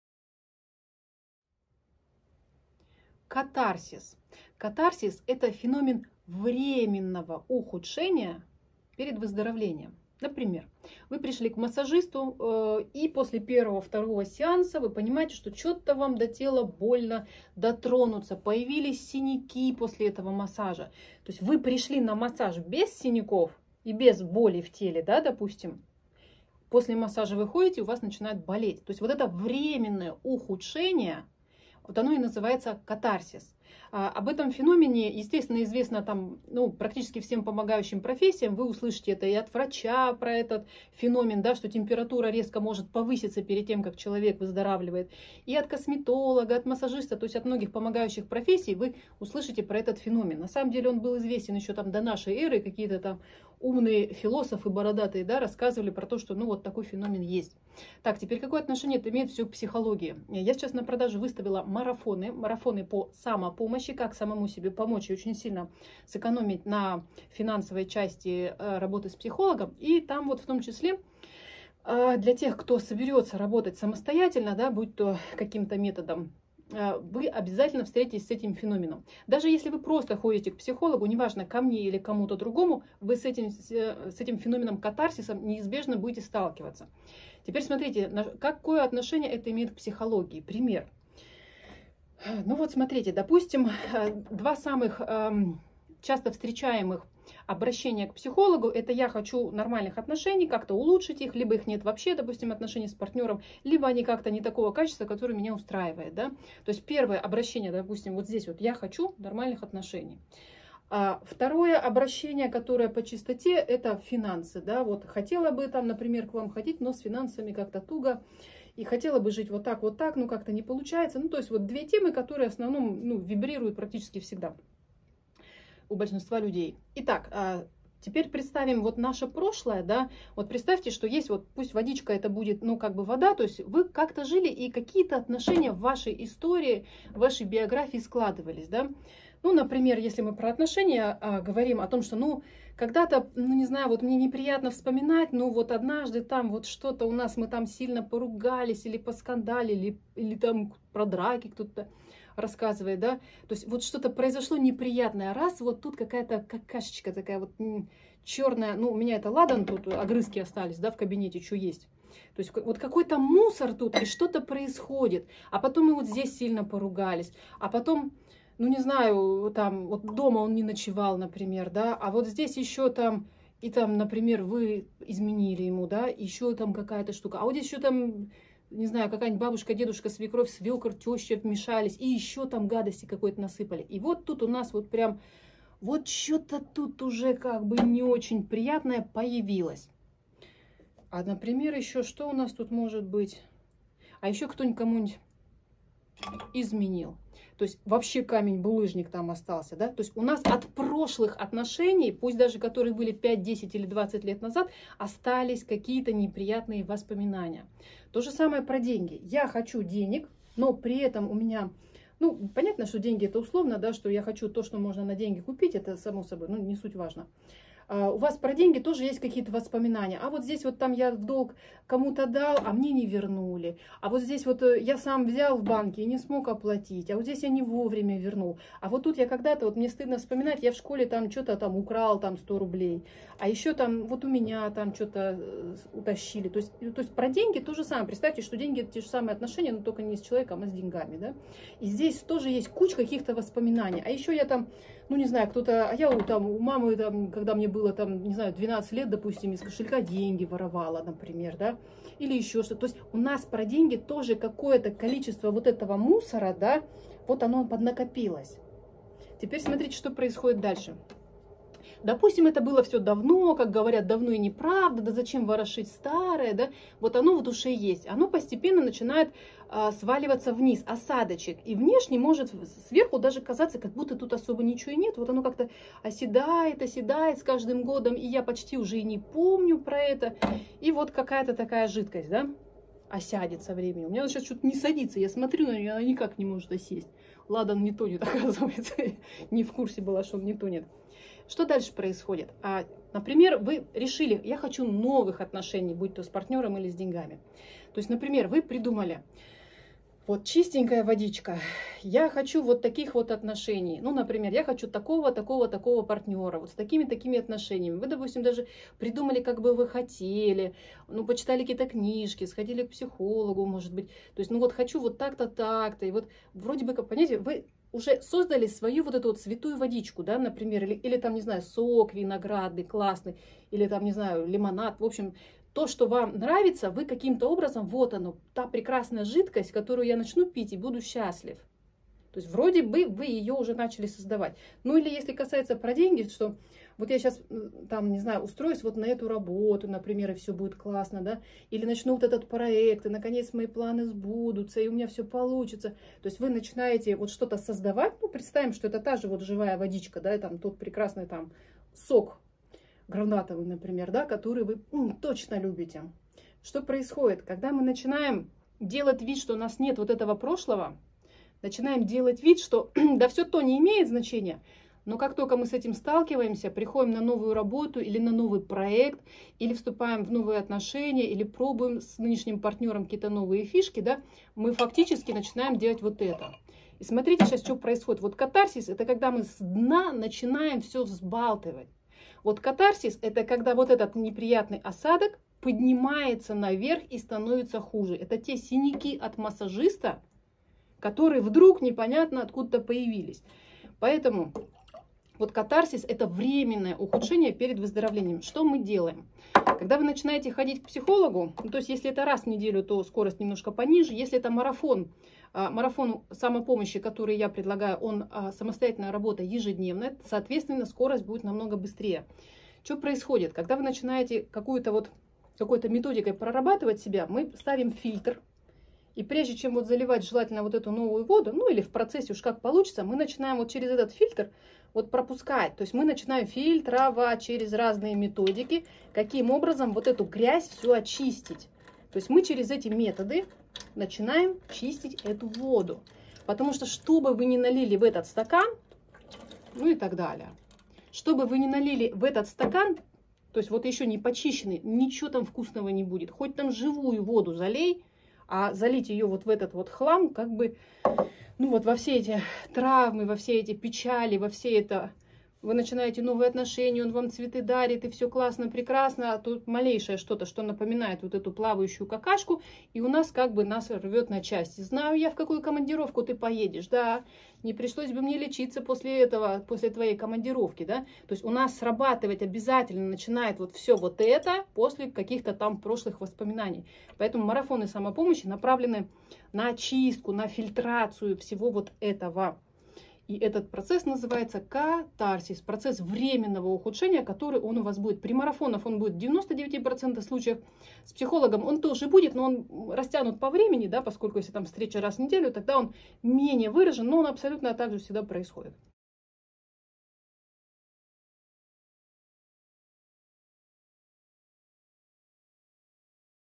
Катарсис
Важно до начала работы над марафоном морально подготовиться к феноменам, которые наверняка будут происходить.

В 99% случаев, когда человек начинает целенаправленно, день за днём прорабатывать одну и ту же тему, эта тема изначально ненадолго может обостриться. Начинает казаться, что всё против вас, что ничего не получается, всё бесполезно, словно пытаясь сбить вас с пути.

Например, женщина начинает отрабатывать тему «Страх перед властными органами», страхи, когда её останавливает полиция на дороге, когда вызывают в налоговую и тому подобное. И именно в период работы над этим марафоном её начинают останавливать ДПС на дороге по 3 раза в неделю, хотя раньше это было примерно раз в полгода.

Её начали донимать настырные рекламные звонки консалтинговых фирм, которые в надежде заполучить нового клиента запугивают, что если она сейчас не откроет с помощью их услуг ООО или ИП, то у неё непременно вскоре начнутся большие проблемы с налоговой. И тому подобное.

В психологии этот феномен временного ухудшения в процессе выздоровления называют катарсис. О данном явлении вам также может рассказать любой врач, массажист, косметолог и другие специалисты помогающих профессий.

Причём, любопытный момент, что катарсис может начаться даже не с того момента, как вы начинаете работать над темой, а с того дня, когда только ещё ПРИНЯЛИ РЕШЕНИЕ проработать эту тему.

Этот феномен был известен ещё до нашей эры, и его описание встречается у древних философов. У кого-то из классиков есть метафора для расшифровки этого слова «катарсис». Представьте СТАКАН с грязной водой. И вам надо воду очистить, отфильтровать. Сначала осевшую на дне грязь придётся ВЗБОЛТАТЬ.

Так и в Душе человека: тягостные воспоминания часто забываются, оседают на дно до поры до времени. И чтобы почистить воспоминания, к ним надо прикоснуться, разворошить их. Но это ненадолго. Когда вы доведёте марафон до конца, вы будете очень благодарны себе за то, что сквозь этот пыльный ветер шли дальше и вышли на ясное солнечное поле с чистым воздухом.